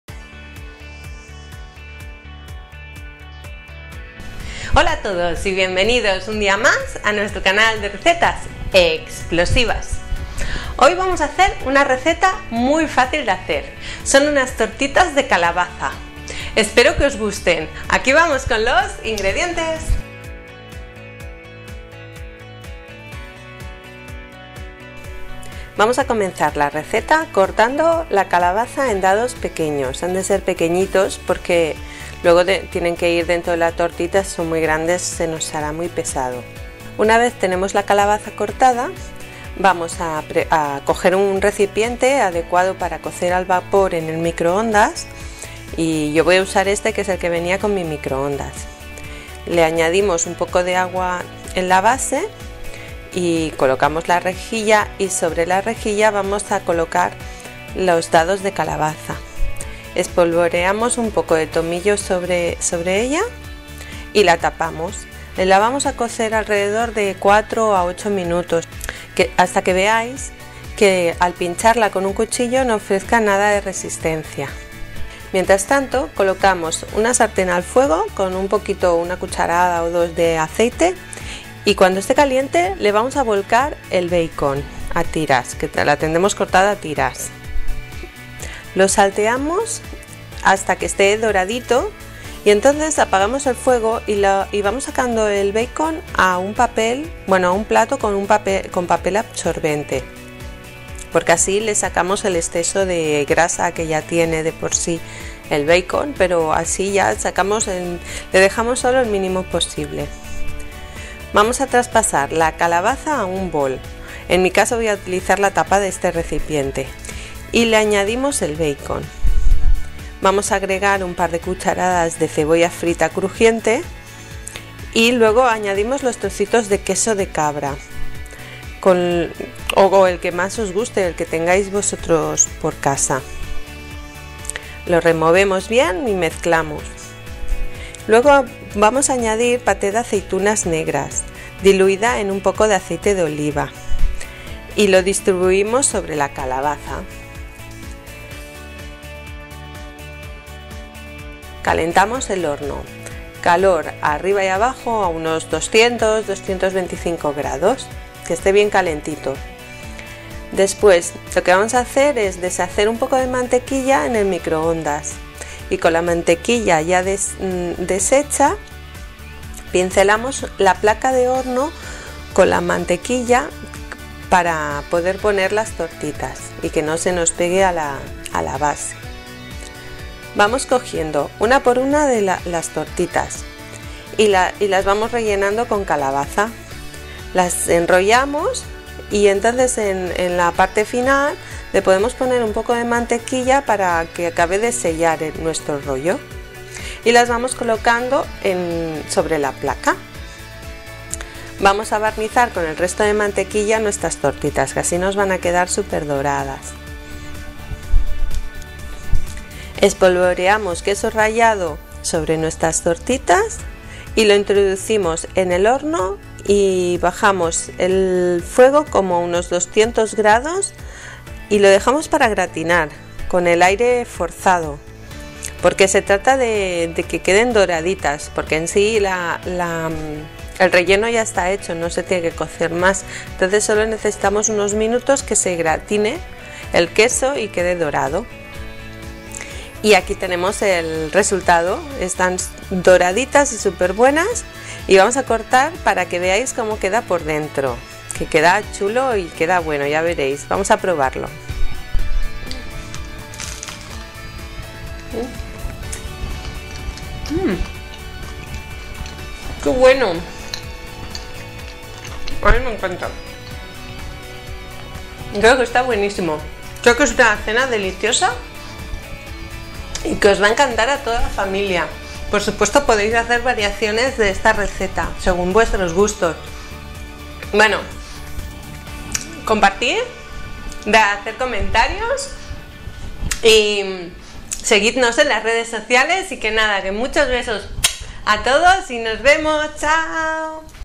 Tortitas o Warps de Calabaza - Super facil - Recetas Explosivas
Cómo hacer una tortitas o wraps de calabaza y queso de cabra. Súper fáciles de hacer y muy sabrosas.

Si te ha gustado la receta. Es muy importante TÚ colaboración, para que podamos seguir adelante con este canal. MUCHAS GRACIAS A TODOS!